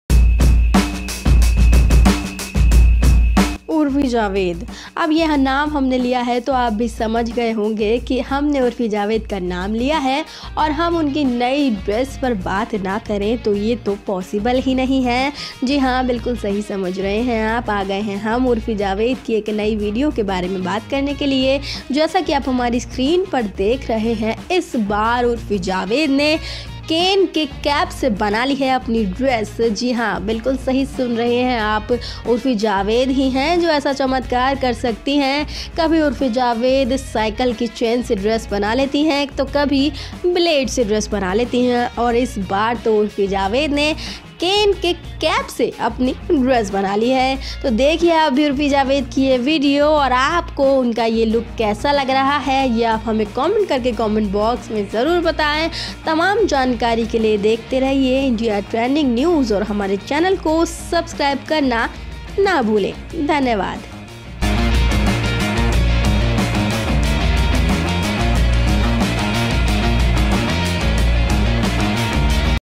Urfi Javed ने बनाई कैन के ढक्कन से Dress, देख Fans के उड़े होश !
Bollywood Planet ( BP ) : Urfi Javed Made Dress From Pepsi Cans And Looks Hot | Shabnam Shaikh And Urfi Javed | Urfi Javed Latest News and Updates

Edited By -